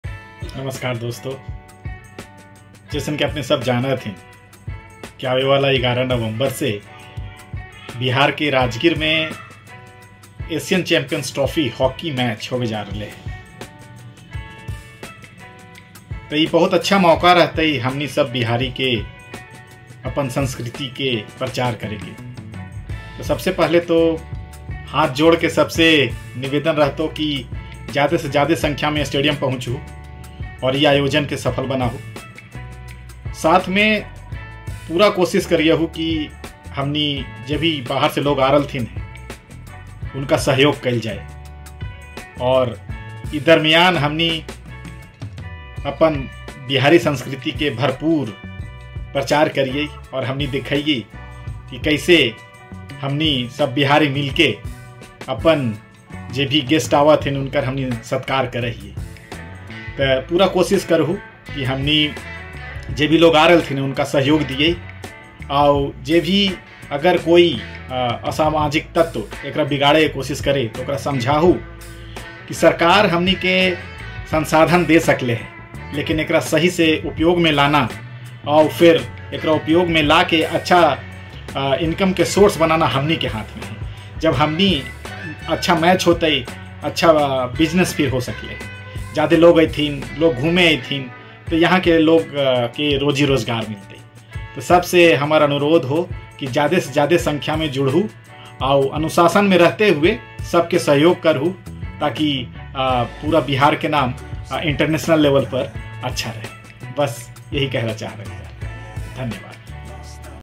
बिहार महिला एशियाई चैंपियंस ट्रॉफी राजगीर 2024 | एकंगर मगही | Ekangar Magahi
राजगीर में होवे वाला "बिहार महिला एशियाई चैंपियंस ट्रॉफी राजगीर 2024" के सफल बनावे के जिम्मेवारी हमनी सब पर हई। आवs सब कोई मिल के एकरा एगो यादगार टूर्नामेंट बनावल जाये। साथ ही साथ इ भी देखावल जाये की कैसे हमनी ला "अतिथि देवो भवः" सार्थक हई।